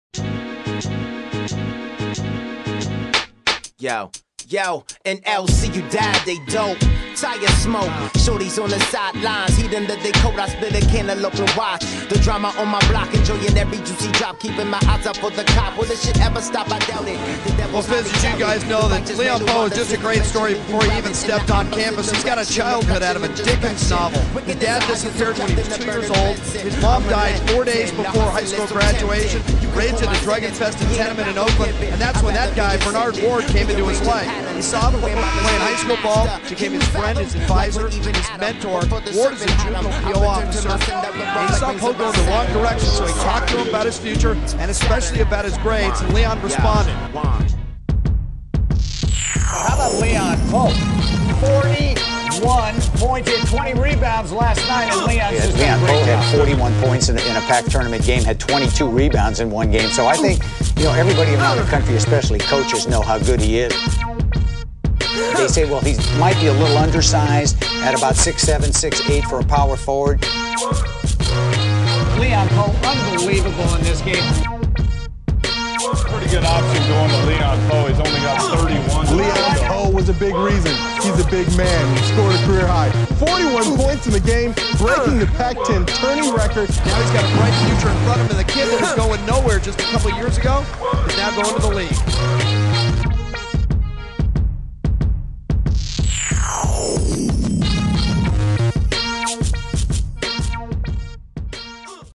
The Show: The Story of Leon Powe
A video that was produced for Cal Berkeley start Leon Powe. This is just an updated version with audio and video from recent games and news reports.